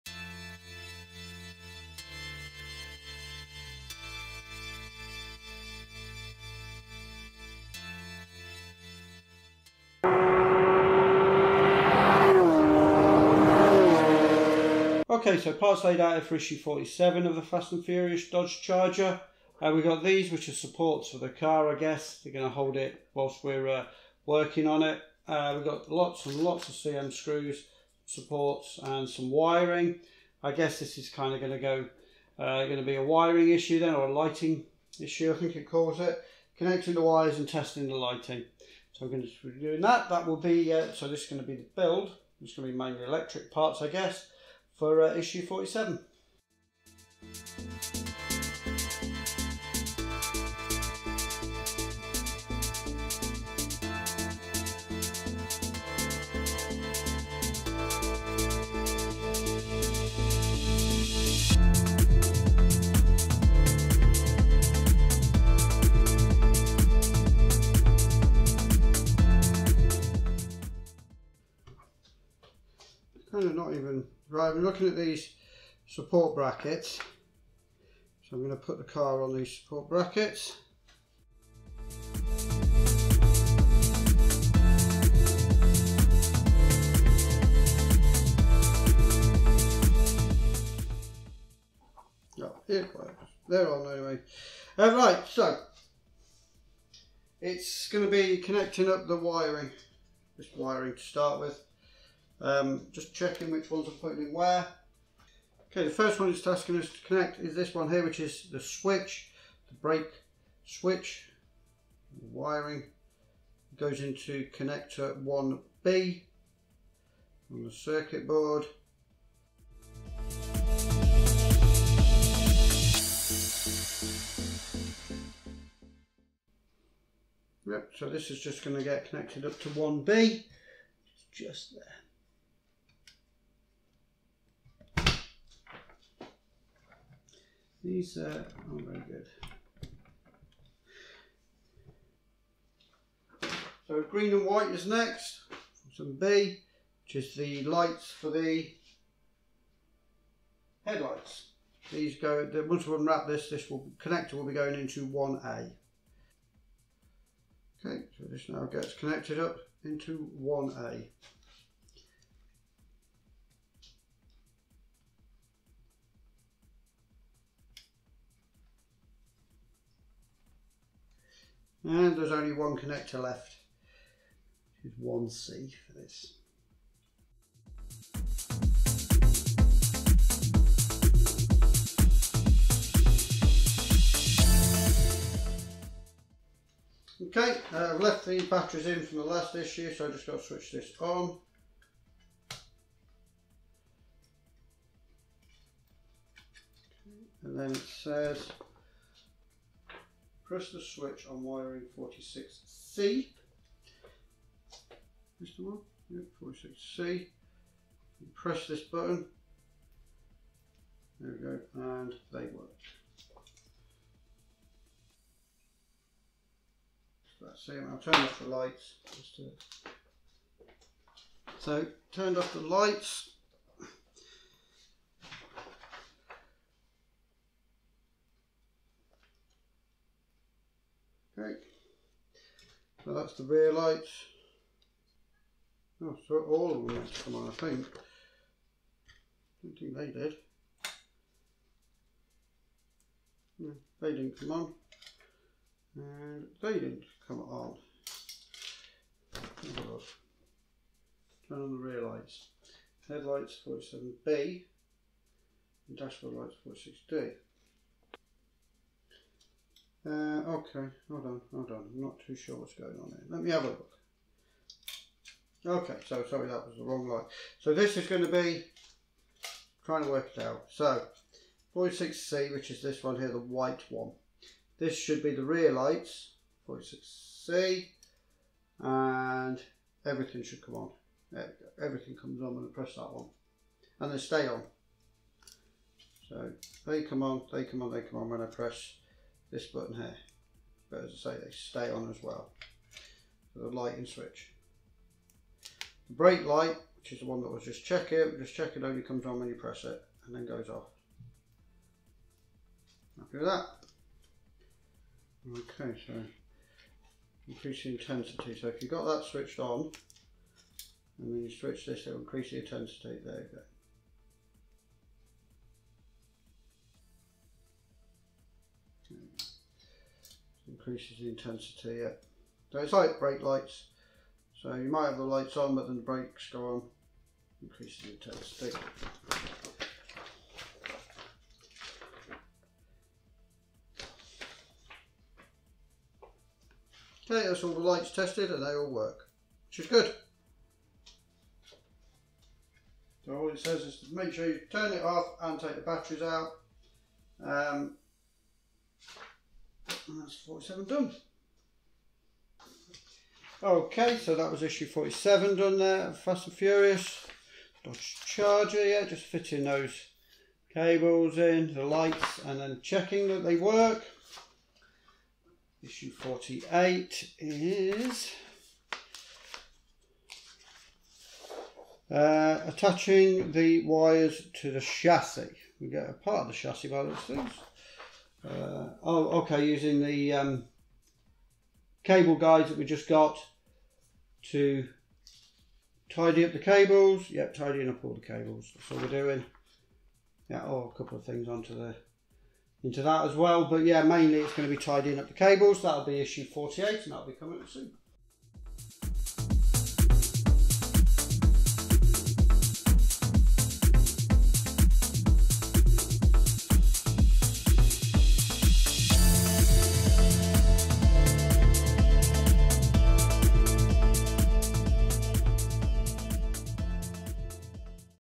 Build the Fast & Furious Dodge Charger R/T - Issue 47. A 1/8 Scale movie car build by FanHome
In this issue I am fitting some more control wires and LED lights, I am also testing the lights.

Please if you can, come and join me as I build up this model.